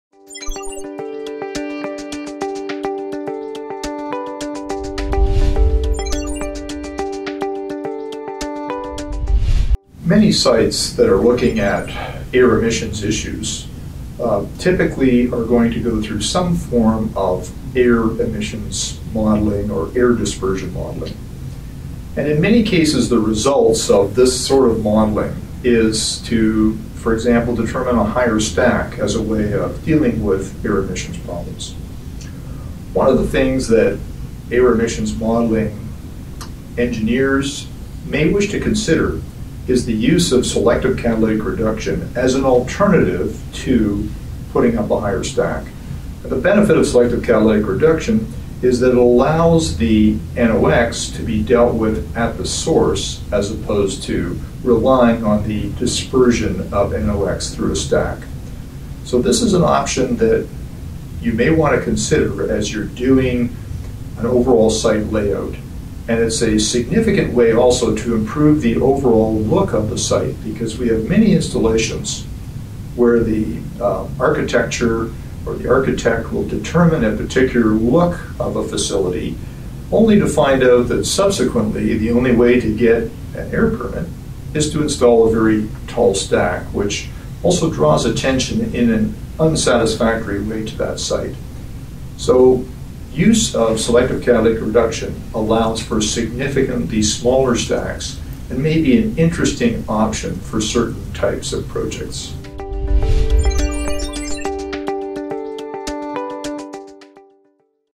Selective Catalytic Reduction (SCR) to Reduce Stack Size
Selective Catalytic Reduction (SCR) technology is often used to reduce the stack height of a building, improving the overall architectural appearance.   This technology reduces the source emissions for a generator greatly assisting the atmospheric dispersion modelling